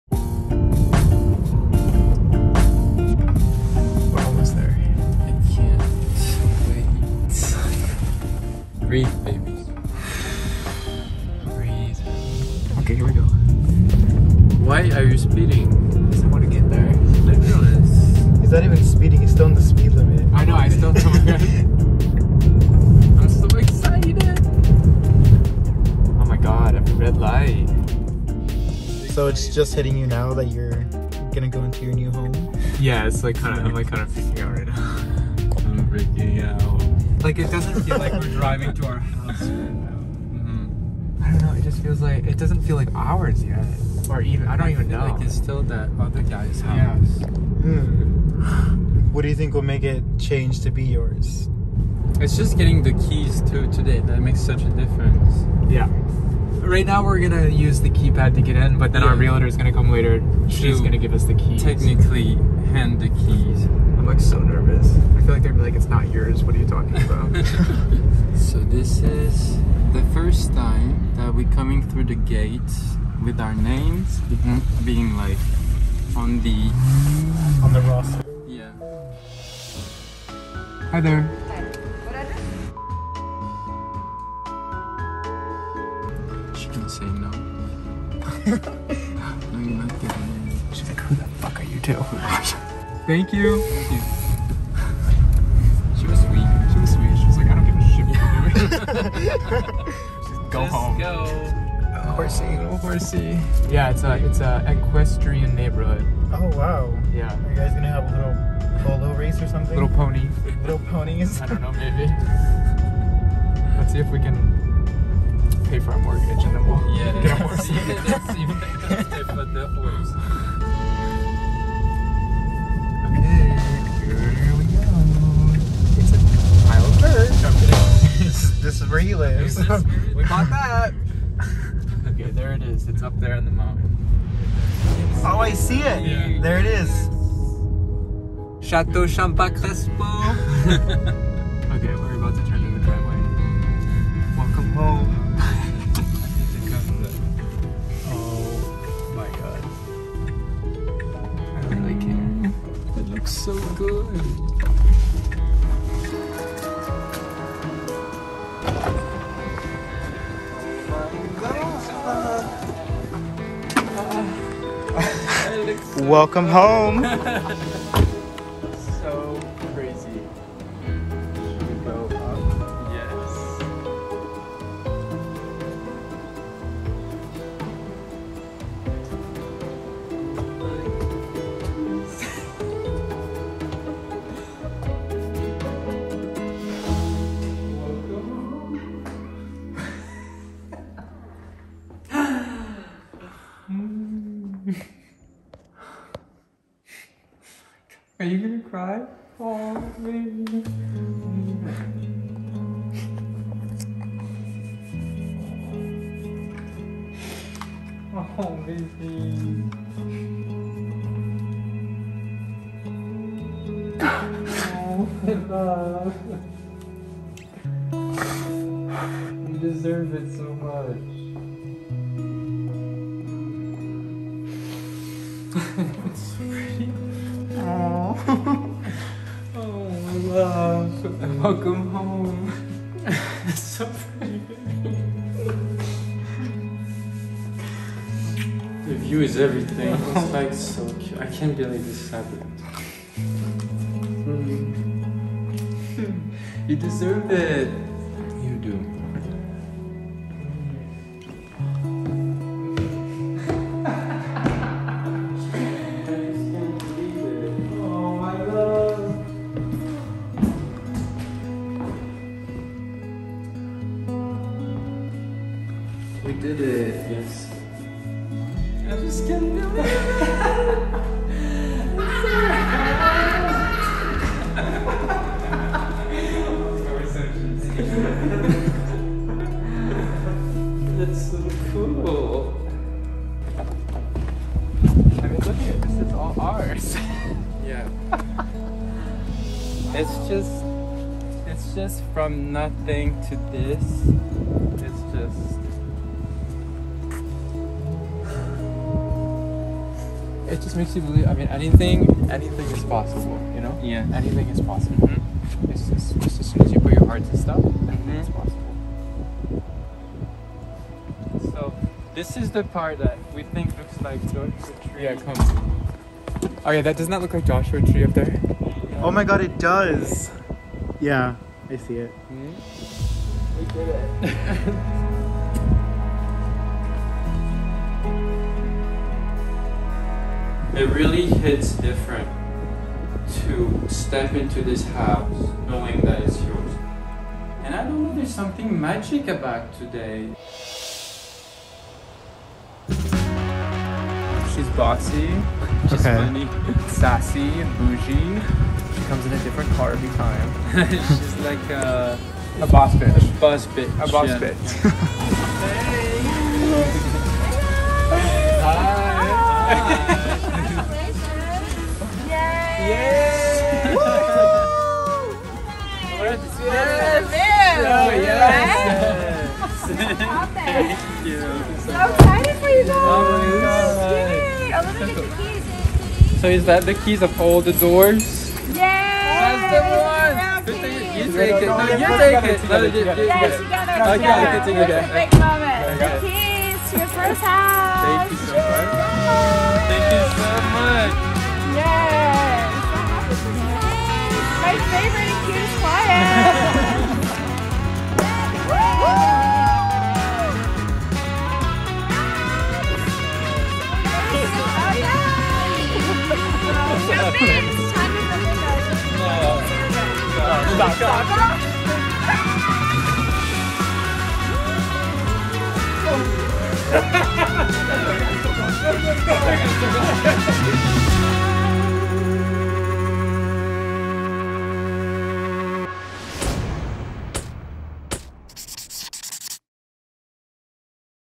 the first step into our new home | Nicky and Pierre
Three weeks ago, we bought our first house together here in Los Angeles. This was the moment when the house officially became ours and we were able to walk into it as the new owners. We're so grateful to be able to show these moments to you and can't wait to share more. It's the beginning to so many new and exciting memories! We love you.
-N&P

shot by Perry Picasshoe @art_p3rry
edited by Irene Young @her.magick